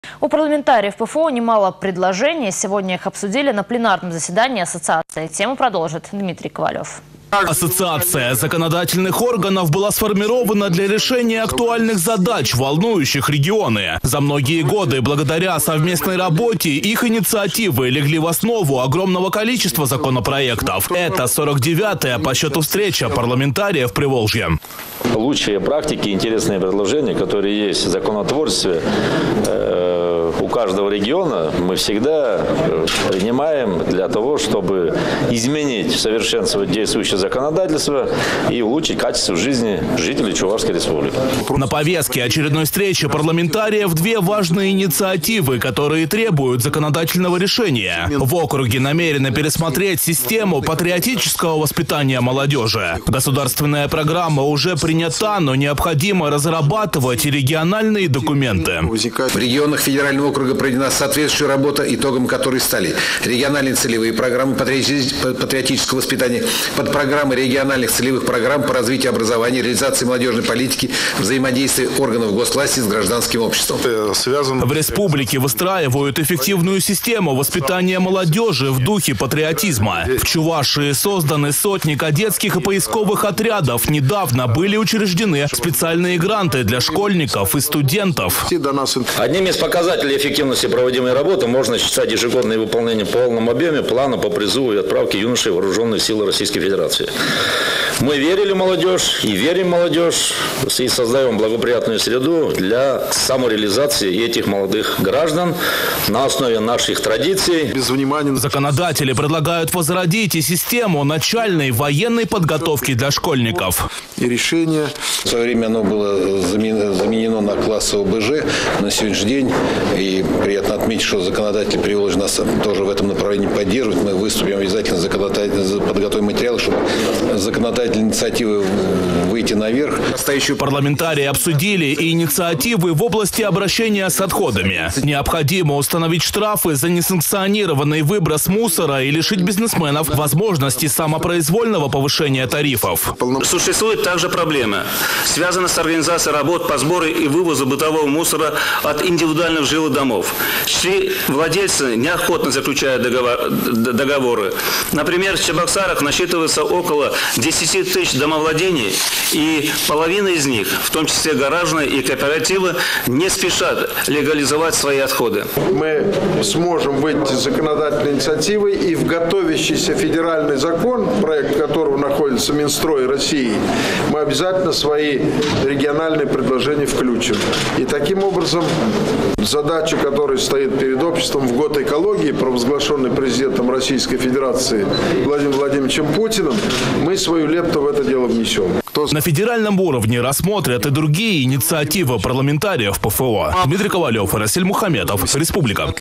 Ассоциация законодателей
От законодательных органов зависит как будут развиваться российские регионы. У парламентариев ПФО немало предложений, сегодня их обсудили на пленарном заседании ассоциации. Тему продолжит Дмитрий Ковалёв.